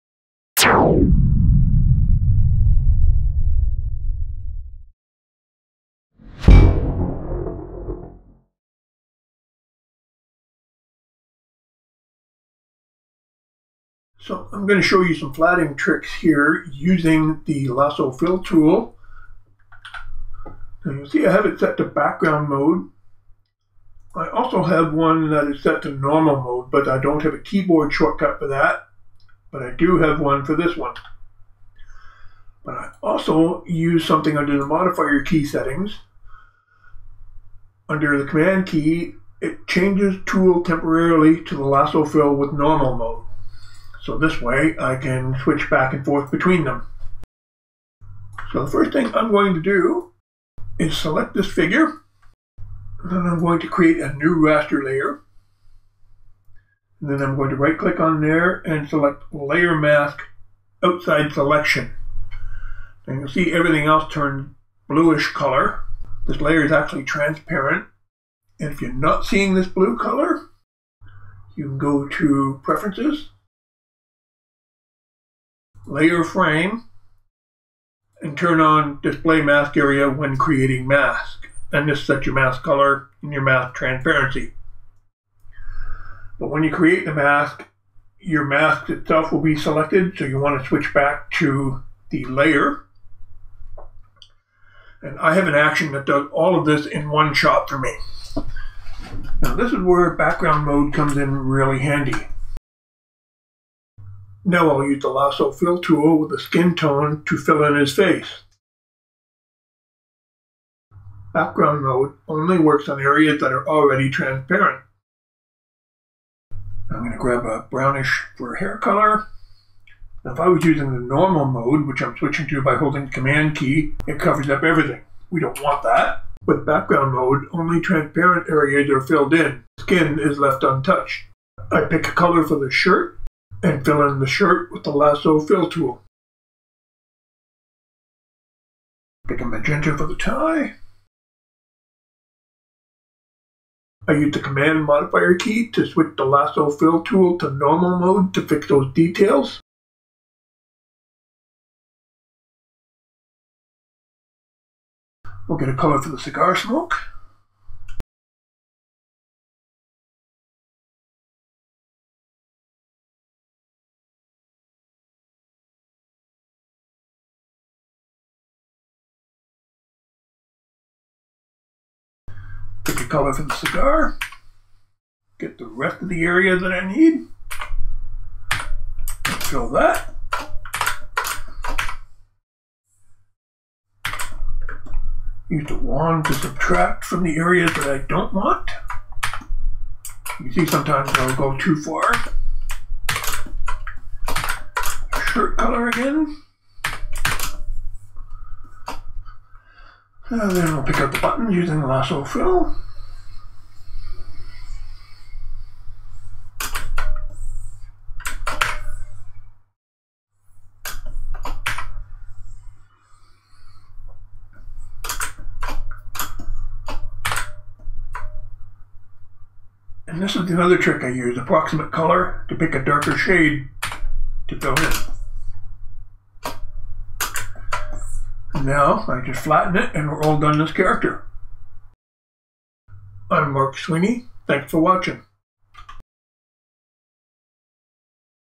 Clip Studio Paint Flatting Tips - Lasso Fill Tool
Use the Lasso Fill Tool in Background mode for faster flatting, and using the Modifier Key setting to temporarily switch to another tool.